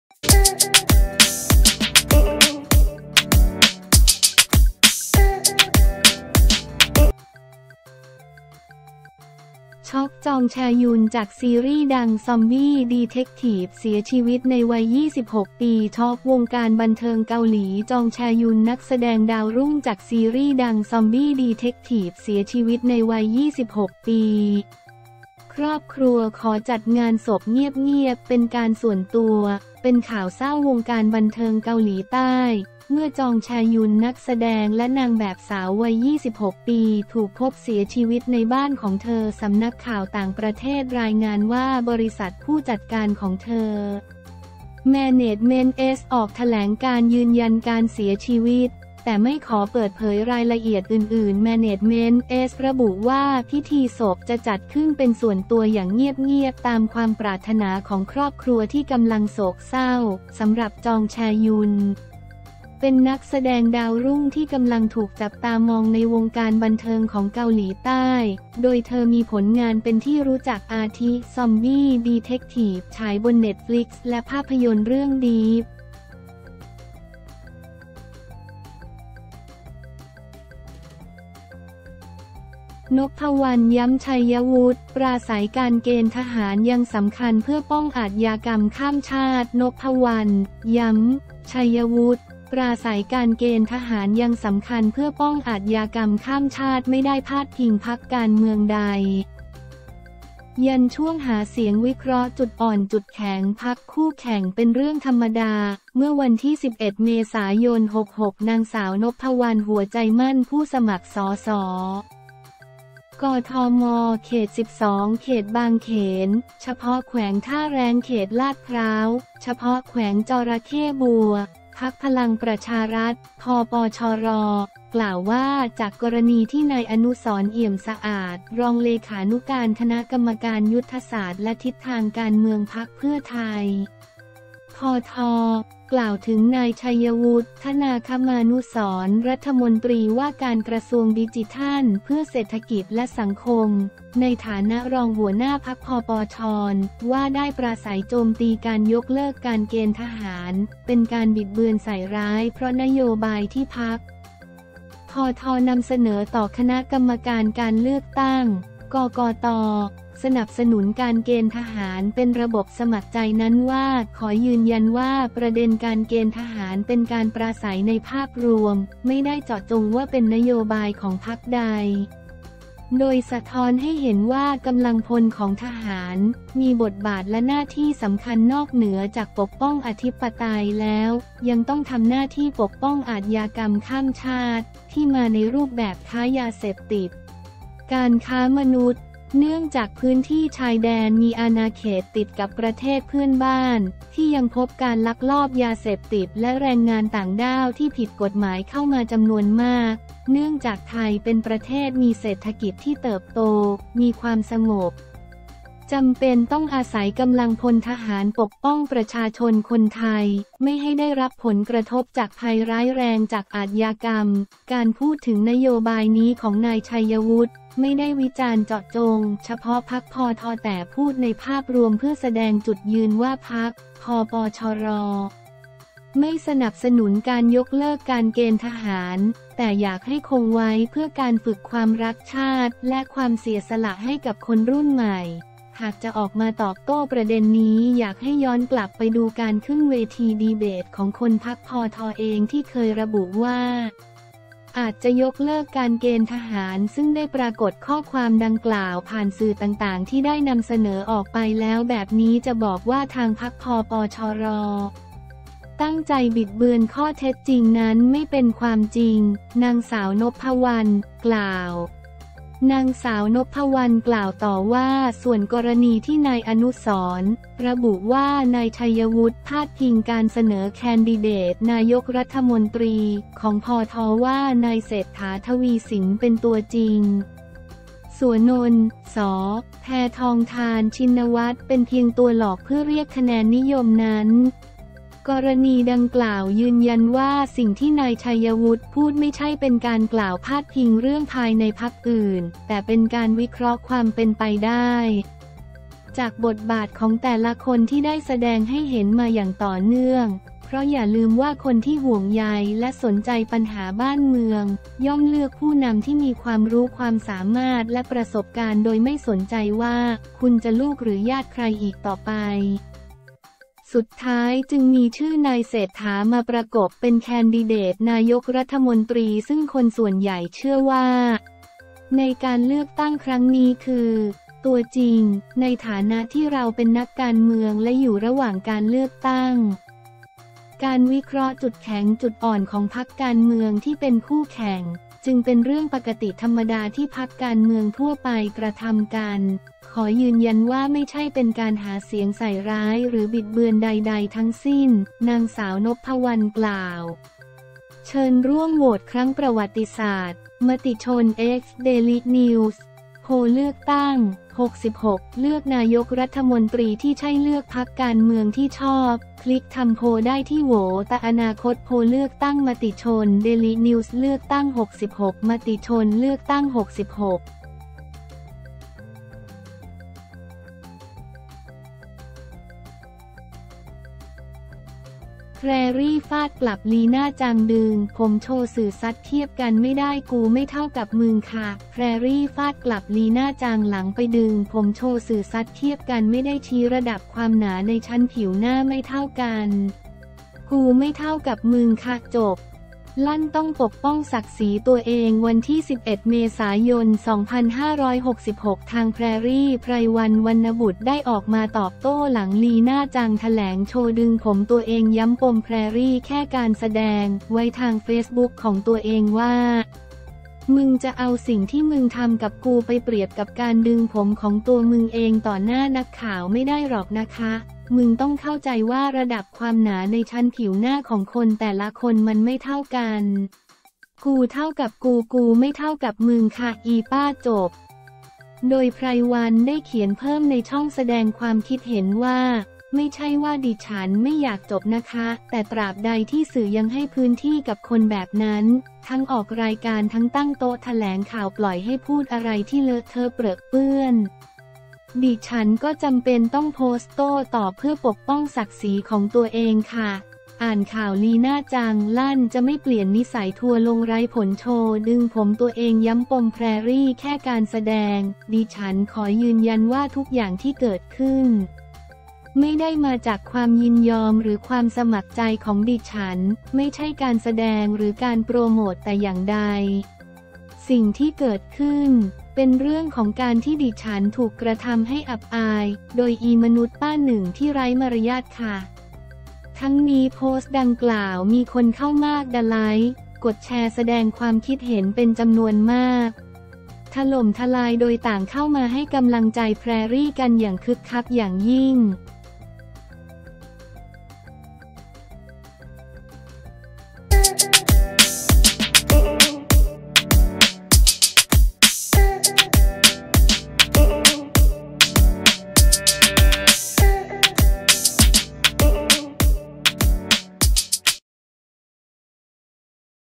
ช็อก จองแชยุล จากซีรีส์ดัง Zombie Detective เสียชีวิตในวัย 26 ปี
ช็อกวงการบันเทิงเกาหลี จองแชยุล นักแสดงดาวรุ่ง จากซีรีส์ดัง Zombie Detective เสียชีวิตในวัย 26 ปี ครอบครัวขอจัดงานศพเงียบๆเป็นการส่วนตัว

เป็นข่าวเศร้าวงการบันเทิงเกาหลีใต้ เมื่อ จอง แช-ยุล นักแสดงและนางแบบสาว วัย 26 ปี ถูกพบเสียชีวิตในบ้านของเธอ สำนักข่าวต่างประเทศรายงานว่า บริษัทผู้จัดการของเธอ Management S ออกแถลงการณ์ยืนยันการเสียชีวิต แต่ไม่ขอเปิดเผยรายละเอียดอื่นๆ
Management S ระบุว่า พิธีศพจะจัดขึ้นเป็นส่วนตัวอย่างเงียบๆ ตามความปรารถนาของครอบครัวที่กำลังโศกเศร้า

สำหรับ จอง แช-ยุล เป็นนักแสดงดาวรุ่งที่กำลังถูกจับตามองในวงการบันเทิงของเกาหลีใต้ โดยเธอมีผลงานเป็นที่รู้จัก อาทิ Zombie Detective ฉายบน Netflix และภาพยนตร์เรื่อง Deep